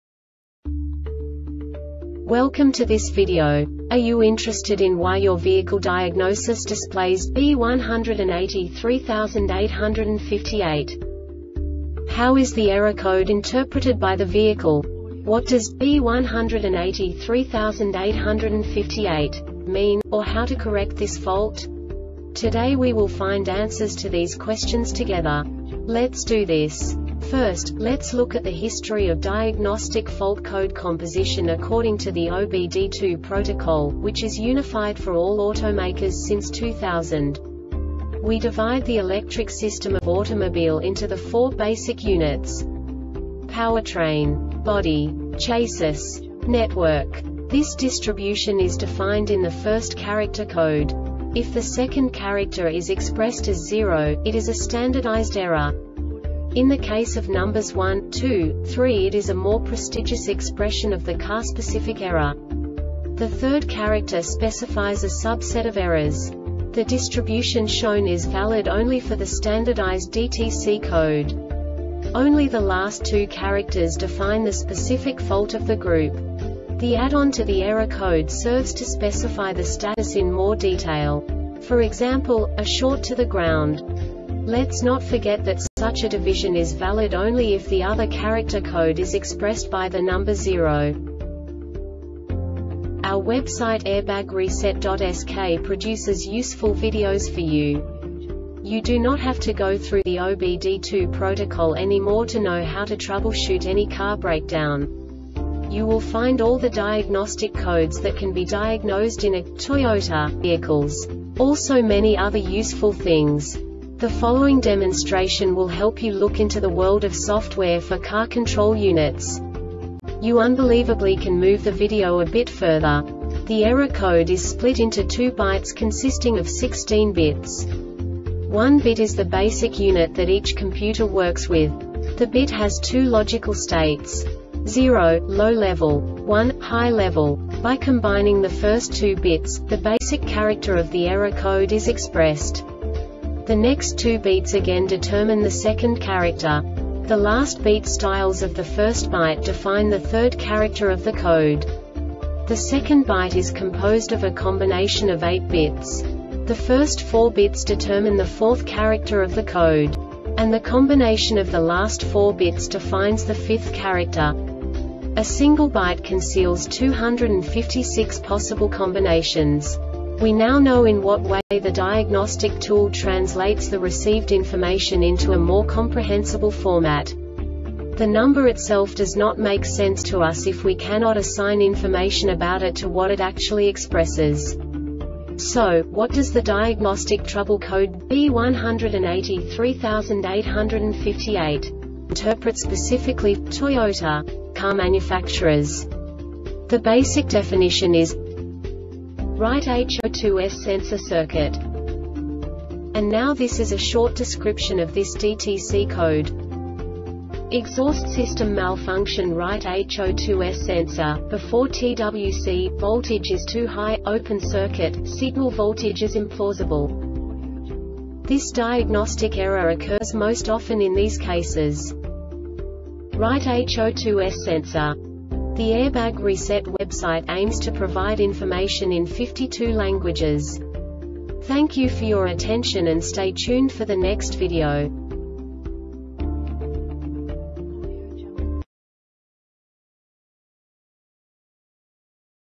DTC Toyota B1838-58 Short Explanation
Contents:
0:21 Basic DTC analysis according to OBD2 protocol standard.
1:48 Insight into programming
2:58 A diagnostically understandable description of the Diagnostic Trouble Code
3:05 Basic error definition

Make:
Toyota
Code:
B1838 58
Definition:
Right HO2S Sensor Circuit
Description:
Exhaust system malfunction.Right HO2S sensor,before TWC, : - Voltage is too high.- Open circuit.- Signal voltage is implausible.
Cause:
Right HO2S sensor